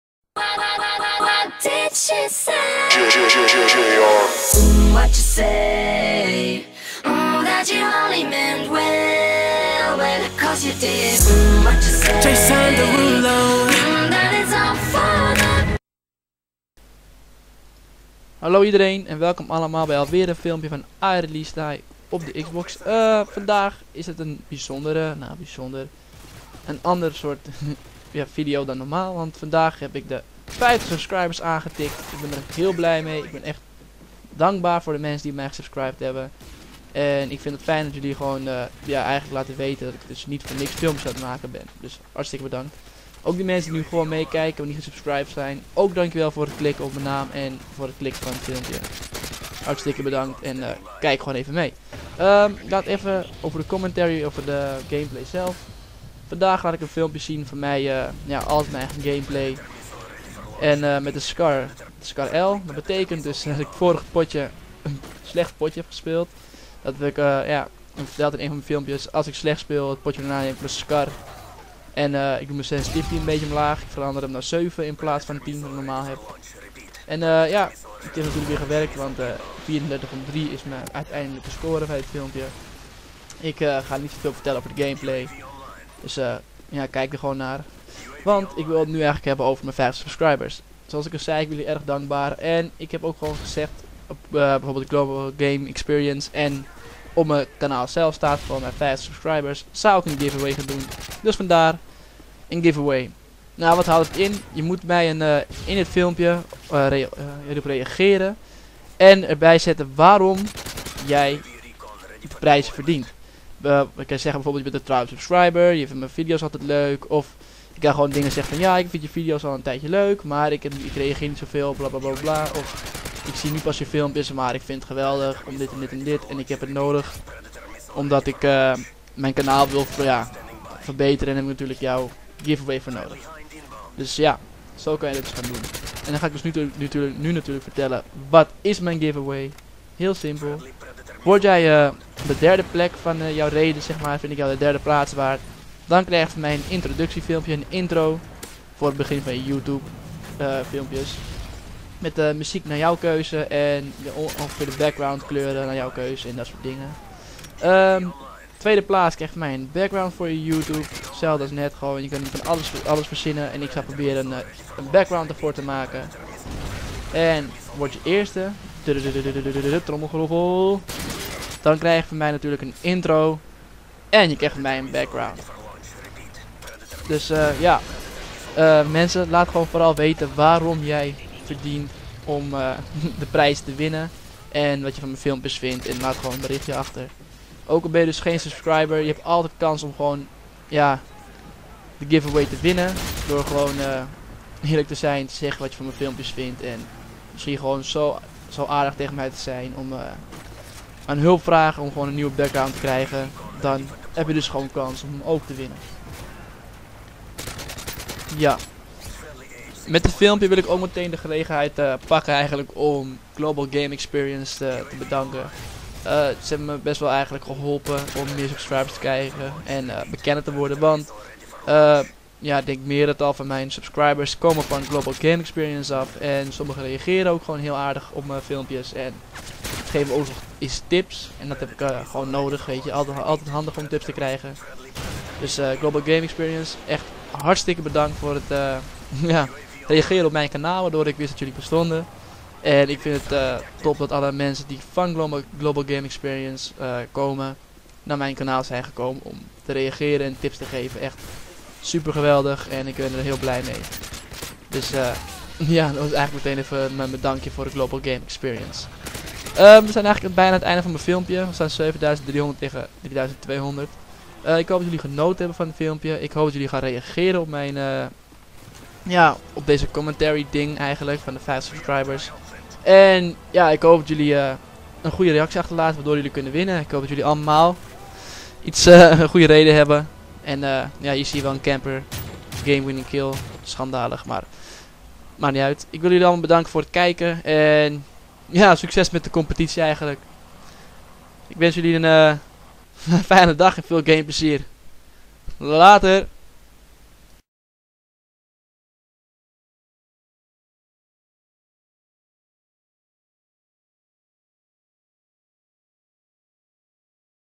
50 SUBS GIVE AWAY COMMENTARY 34-3
Hallo mensen,
Het is eindelijk zo ver! 50 subscribers op mijn kanaal :D
Bedankt hiervoor! en daarom een give away commentary!
Bekijk em en reageer voor de give aways! De uitslag is maandag 19-3.
Veel succes! en nogmaals dank!